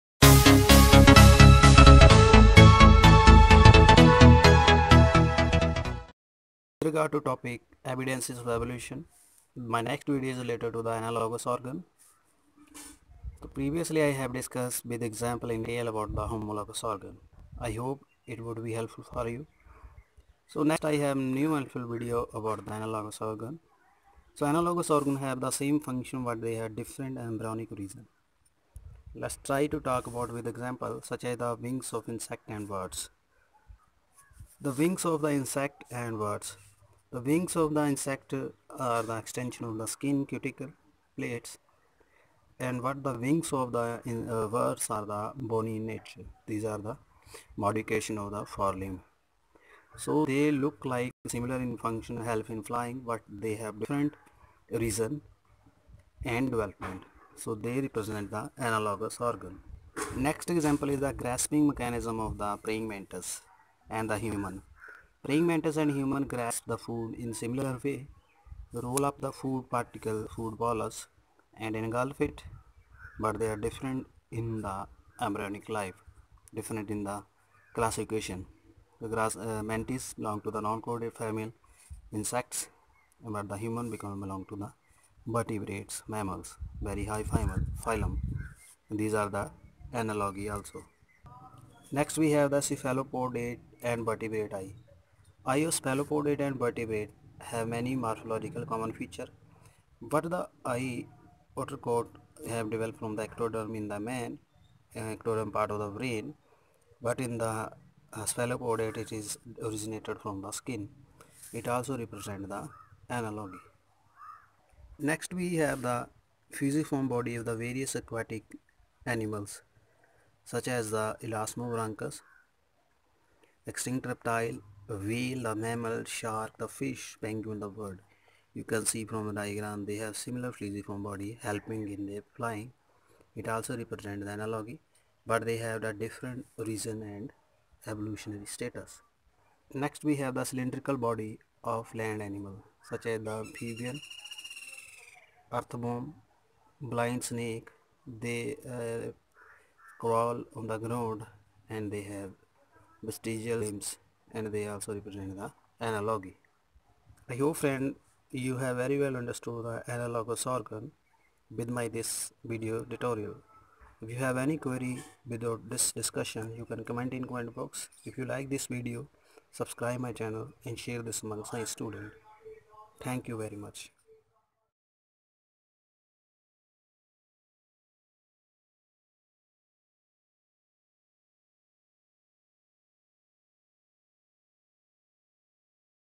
Analogous Organs with examples
Hello students here is my video tutorial on YouTube for you various types of analogous Organs with examples. I hope it will helpful for you.